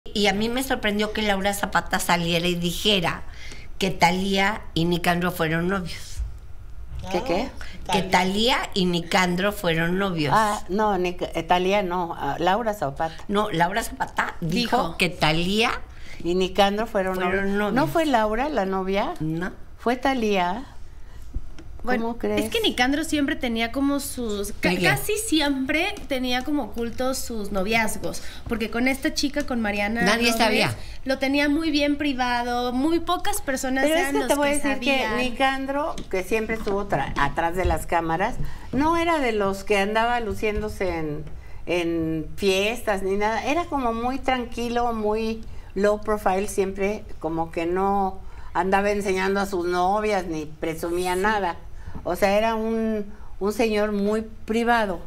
¿Nicandro Díaz y Thalía fueron novios?
Laura Zapata reveló que Thalía y Nicandro Díaz tuvieron una relación amorosa. Lamentó mucho su muerte.

Grupo Fórmula

Canal sugerido

Descarga nuestra